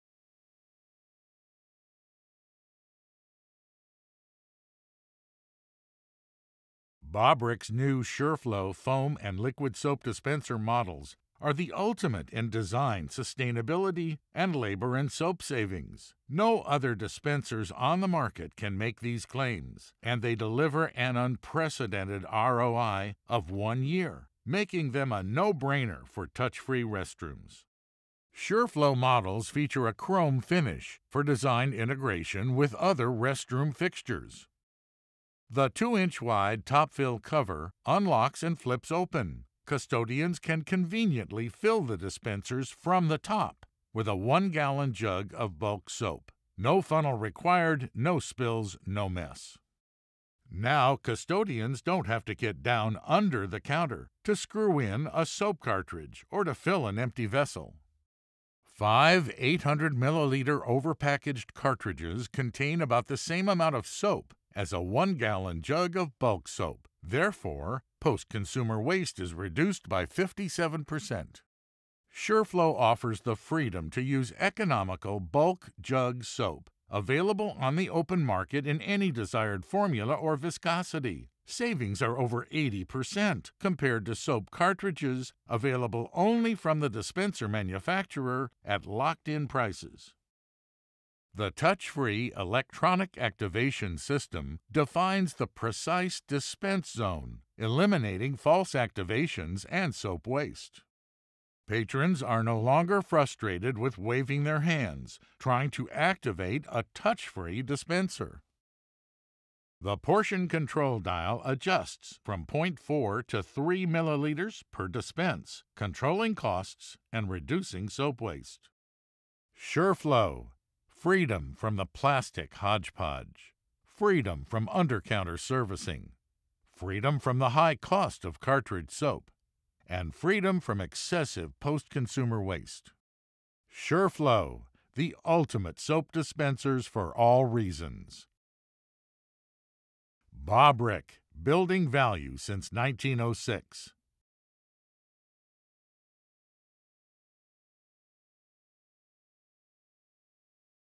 Bobrick Inc. || Sureflo - The Ultimate Soap Dispenser
SureFlo® The ultimate soap dispenser for all
reasons. Automatic, Top-Fill Bulk Liquid and Foam
Soap Dispeners. Top-fill feature eliminates
time-consuming under counter servicing of
cartridges. Bulk soap saves over 80% compared to
proprietary soap cartridges, and reduces post
consumer waste by 57%. Chrome finish provides
design integration with other restroom fixtures.
Electronic sensor defines "dispense zone"
eliminating false activation, dispensers are
adjustable from .4ml to 3ml, conserving soap. ROI
one-year.